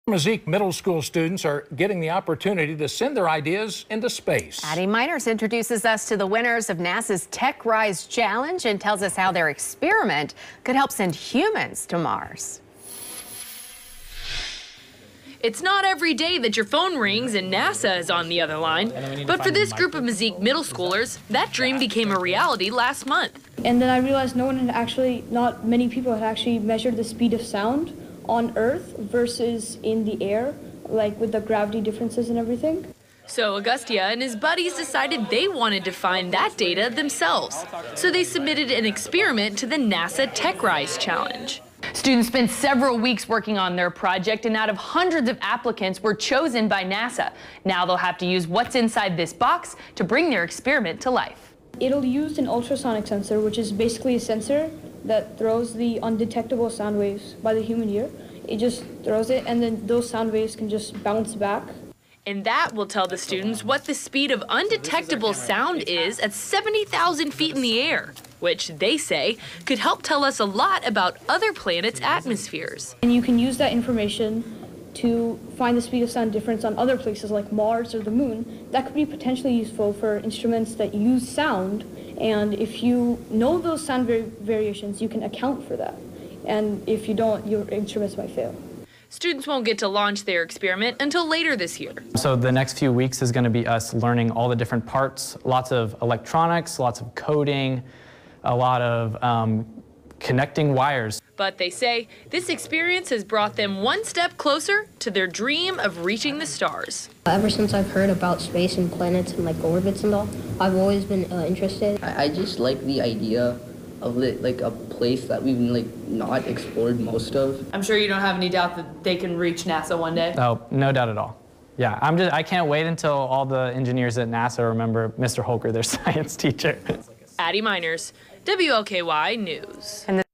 Meyzeek Middle School students win NASA Tech Rise challenge
Myzeek Middle School students win NASA Tech Rise challenge